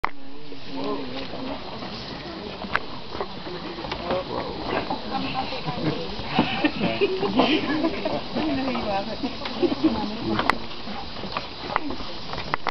Sepilok OrangUtan Rehabilitation Centre
Short video from viewing platfor at Sepilok OrangUtan Rehabilitation Centre in Sabah, Malaysian Borneo.